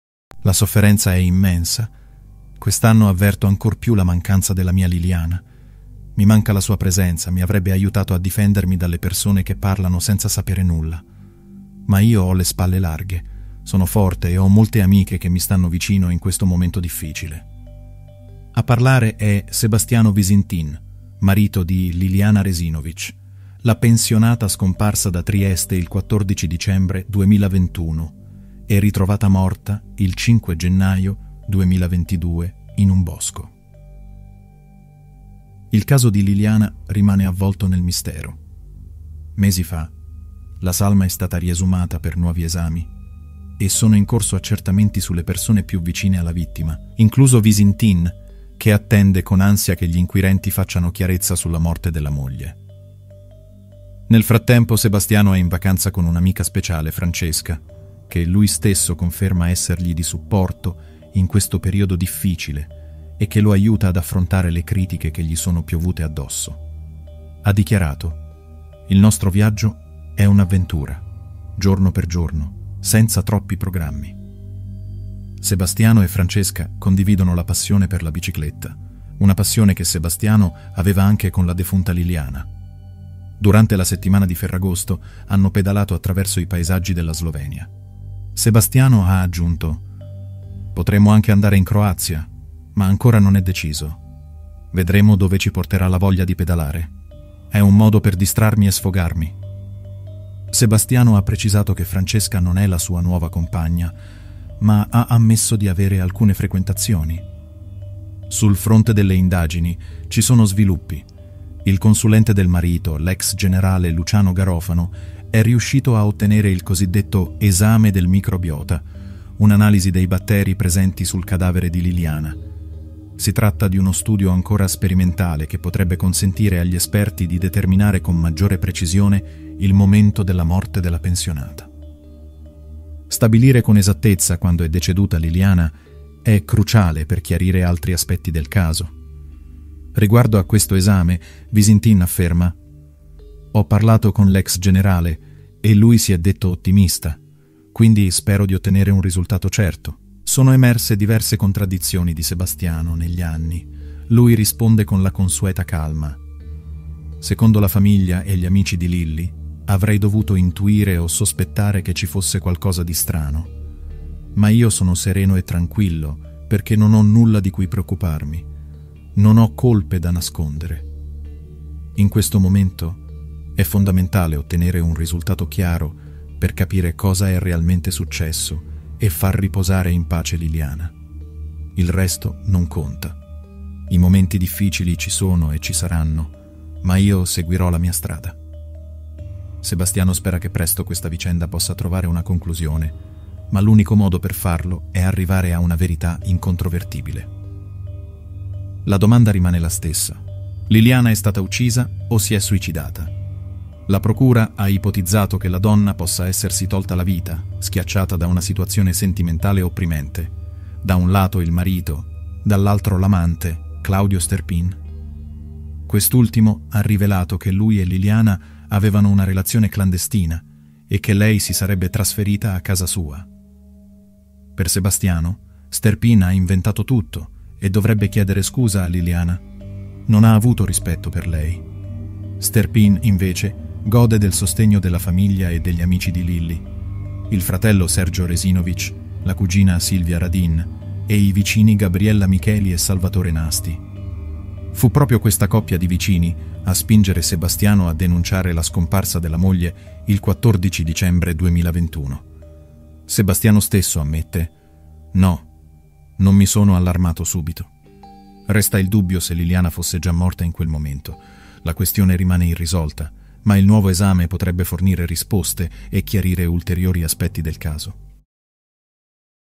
"All'inizio non ero allarmato" - Liliana Resinovich
Il marito di Liliana Resinovich è in viaggio con un'amica speciale.